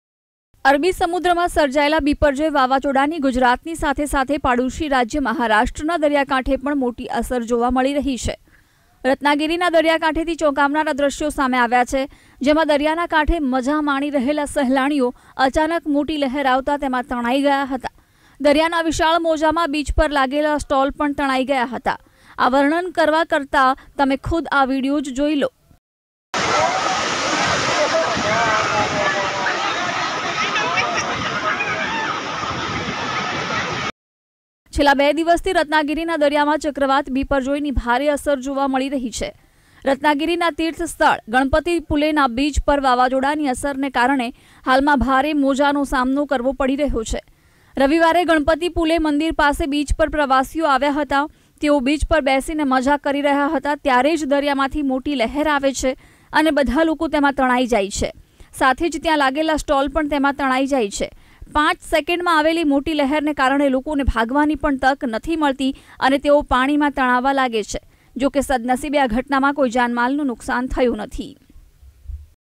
Biporjoy Cyclone: રત્નાગિરીમાં શાંત દરિયાએ 5 સેકન્ડમાં બતાવ્યું રૌદ્ર સ્વરૂપ | VTV Gujarati
VIDEO: રત્નાગિરીમાં શાંત દરિયાએ 5 સેકન્ડમાં બતાવ્યું રૌદ્ર સ્વરૂપ, અચાનક મોટી લહેર આવતા લોકોનું ટોળું તણાયું, ભાગી પણ ન શક્યા